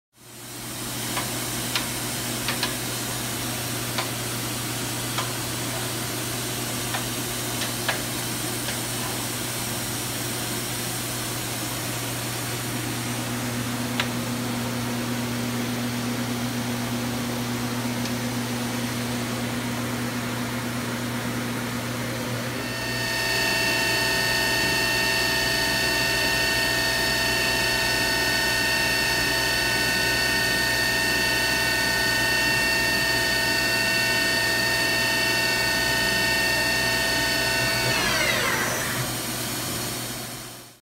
Daewoo DMV 3016L CNC Vertical Machining Center
Check out this Daewoo DMV 3016L CNC VMC for sale on CNCMachines.Net. For more details regarding this Daewoo DMV 3016 L, give us a call at .